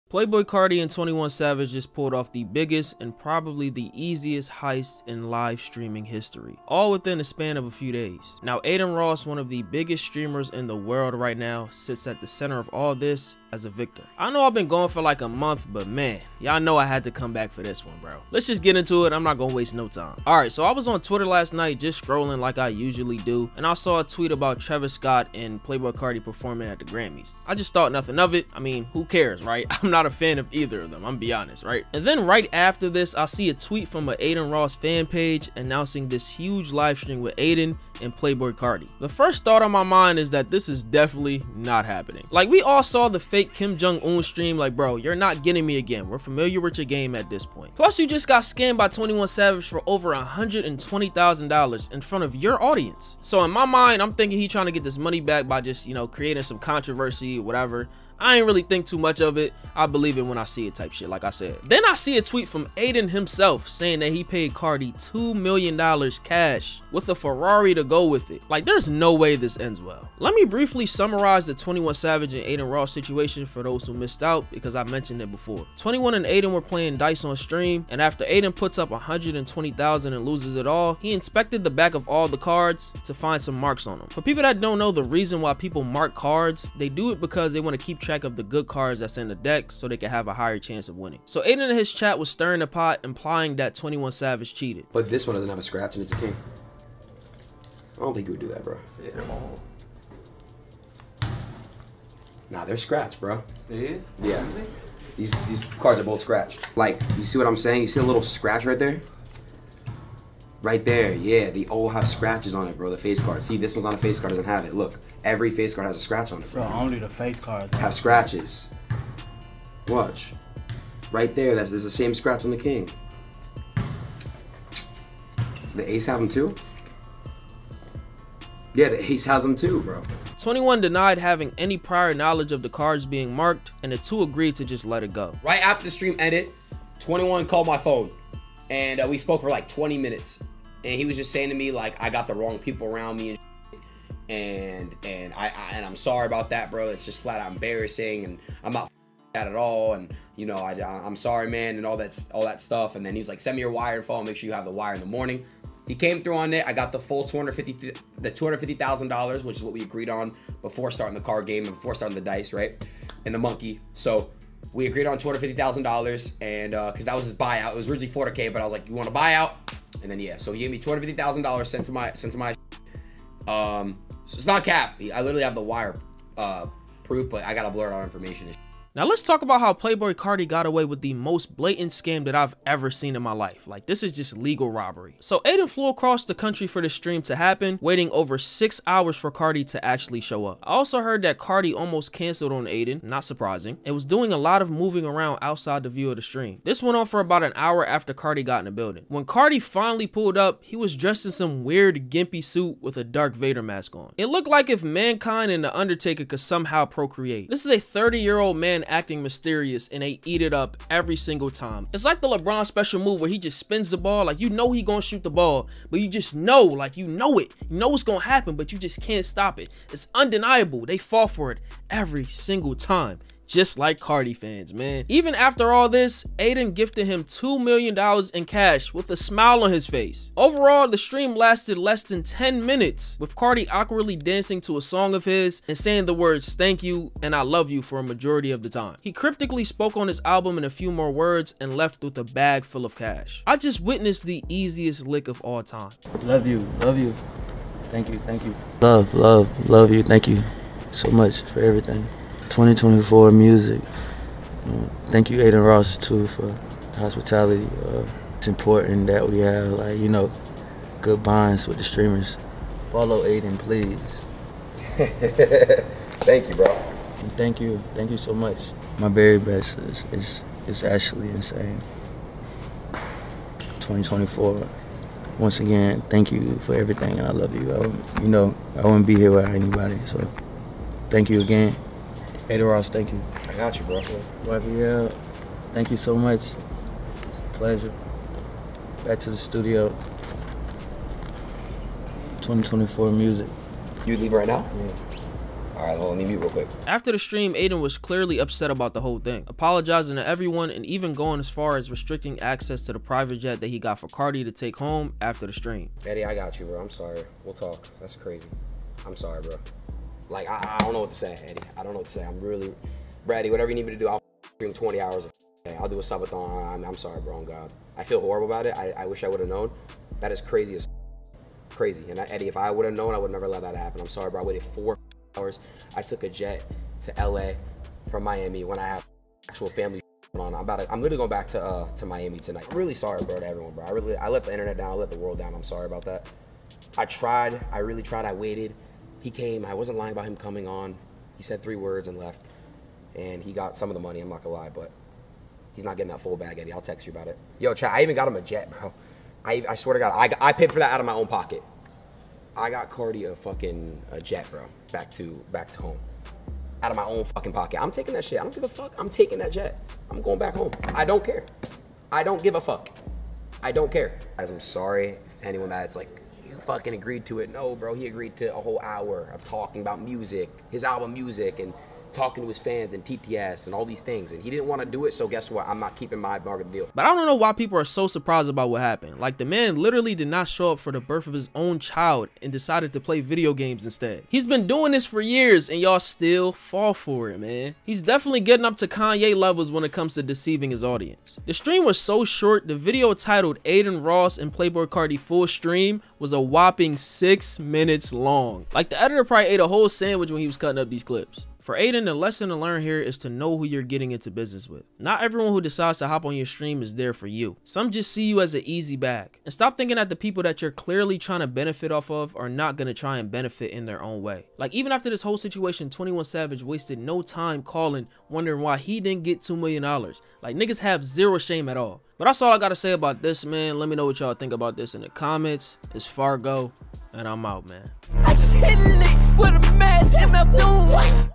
Playboi Carti Just Pulled Off The BIGGEST Scam in Streaming History ($2 Million Dollars)
Playboi Carti and 21 Savage just pulled off the Biggest (and probably the easiest) heist in Streaming History, all within the span of a few days. Now Adin Ross, one of the biggest streamers in the world right now, sits at the center of all this as a victim.